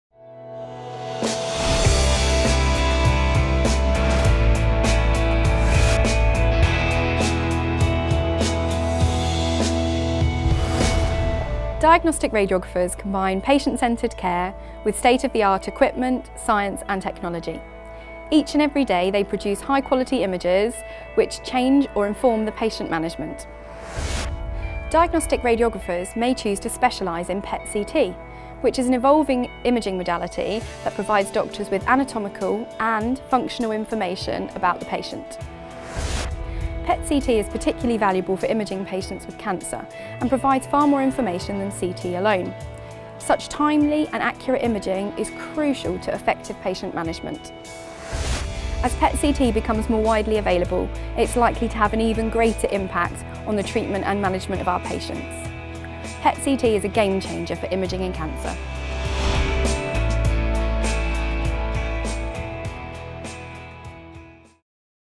A Gamechanger for Diagnostic Radiography
Hollie Hawden, Lecturer in Diagnostic Radiography at the University of Suffolk, discusses a gamechanger in her field of expertise.